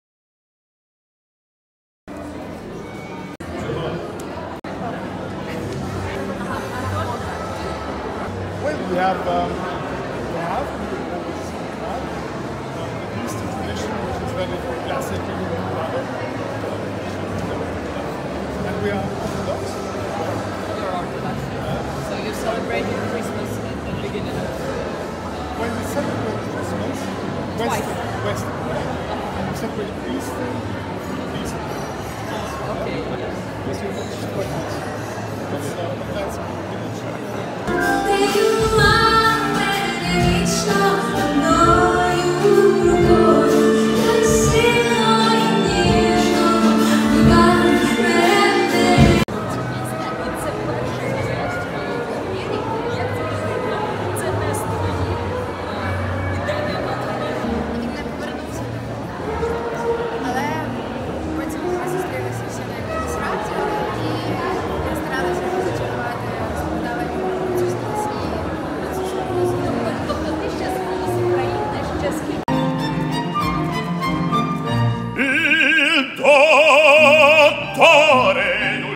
Катерина Осадча завітала на Український бал у Відні
Популярна українська ведуча програми "Світське життя" Катерина Осадча завітала на 5-й Український бал у Відні.